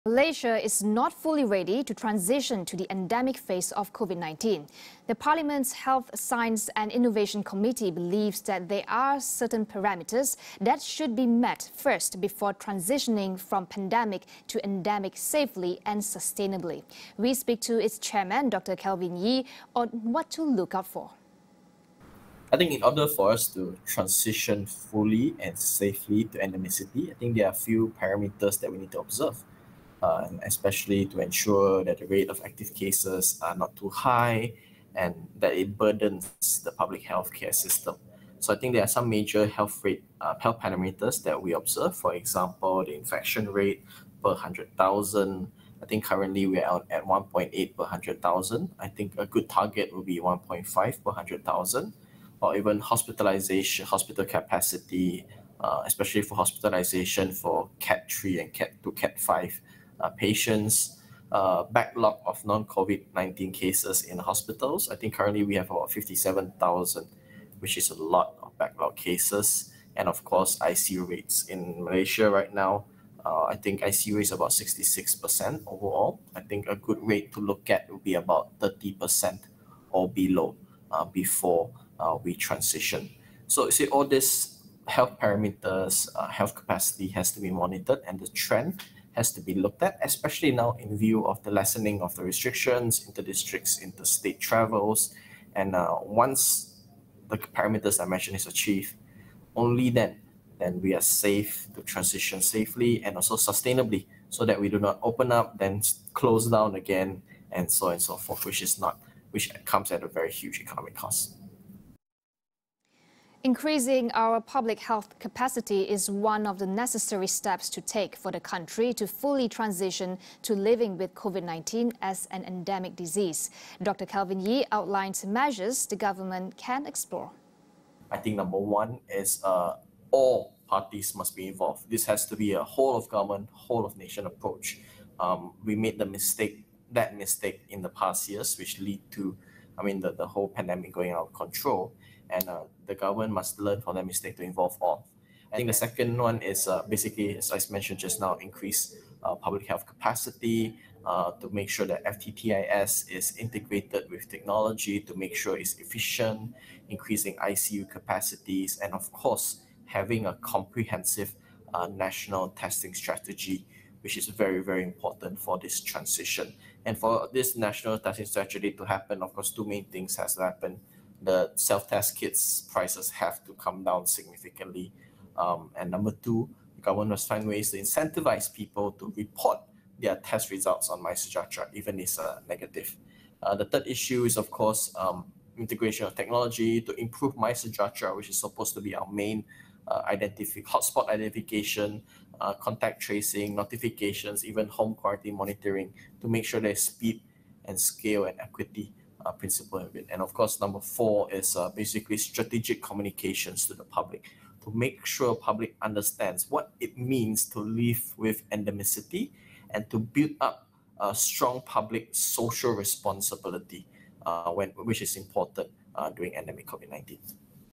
Parliament Committee: M'sia not ready to transition to endemic COVID-19
Health, science and innovation PSC chairman Dr Kelvin Yii outlines public health parameters to meet if M'sia wants to transition to endemic COVID-19 safely and sustainably.